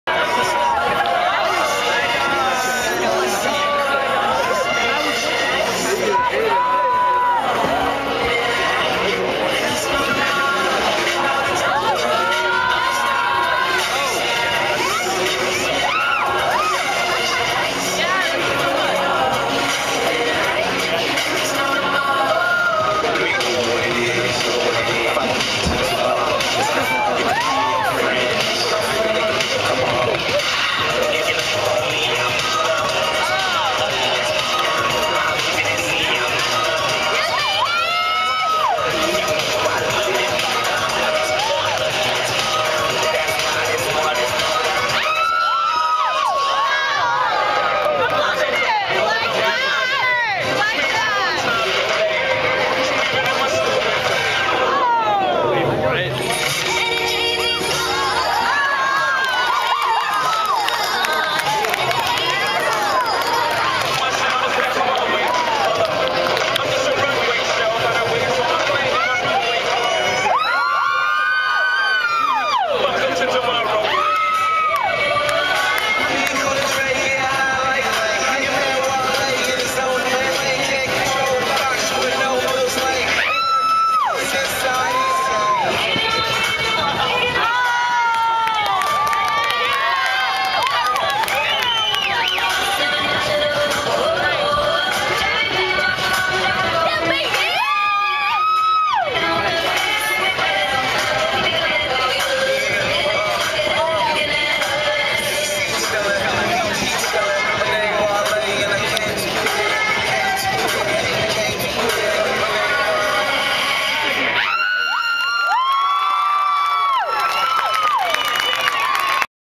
Bentley Craze Homecoming Performance 2009
Bentley's hip hop dance group, Craze, performing at the Homecoming game's halftime, October 24, 2009